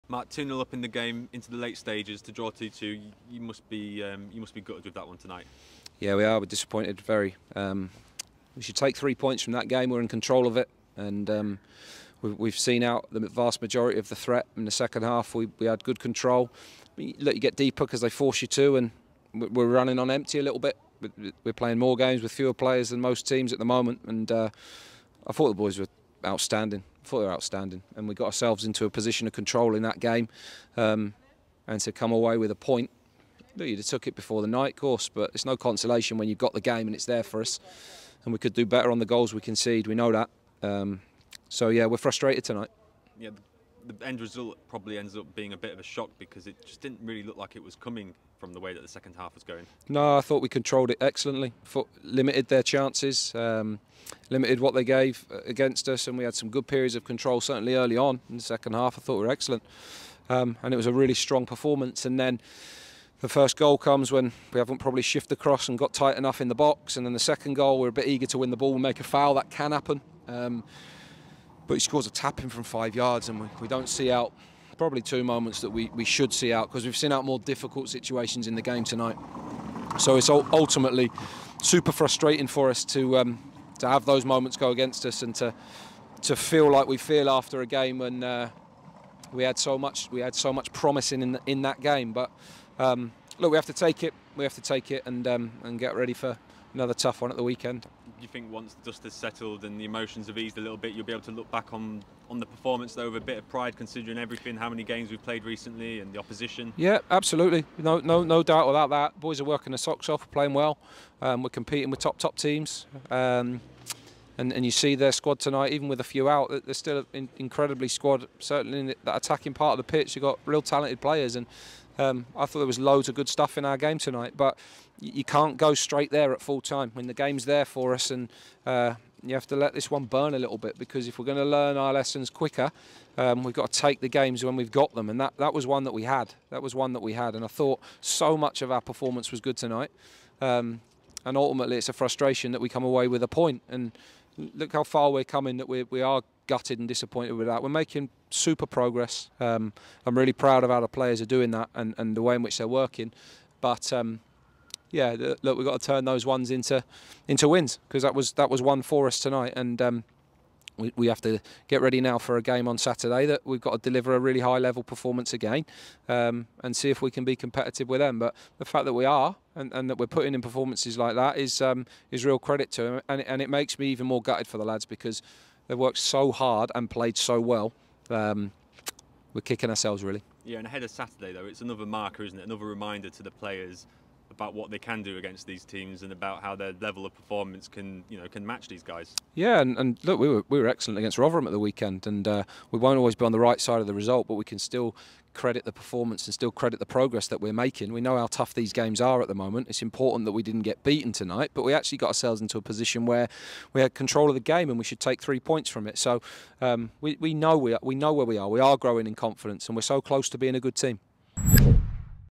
Cambridge United 2-2 Wigan Athletic | Mark Bonner Reaction
Hear from Mark Bonner after the U's 2-2 draw with Wigan Athletic in SkyBet League One.